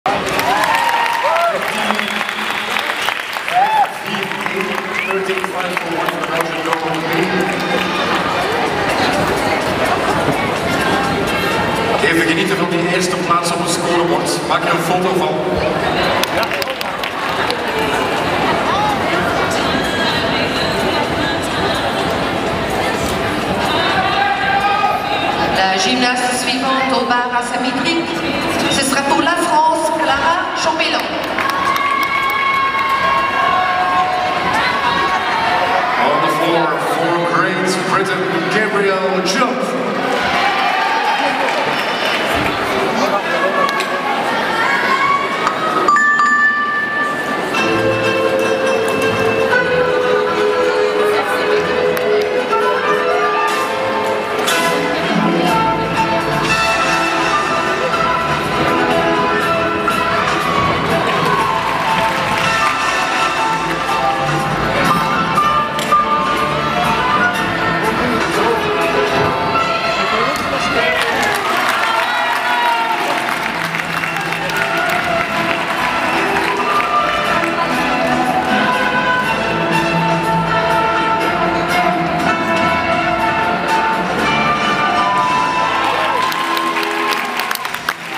Evgeniya SHELGUNOVA RUS, Beam, AA Final, Junior European Gymnastics Championships 2012
Evgeniya SHELGUNOVA RUS on Beam in the All-Around Final of the Junior European Women's Artistic Gymnastics Championships, Brussels, 2012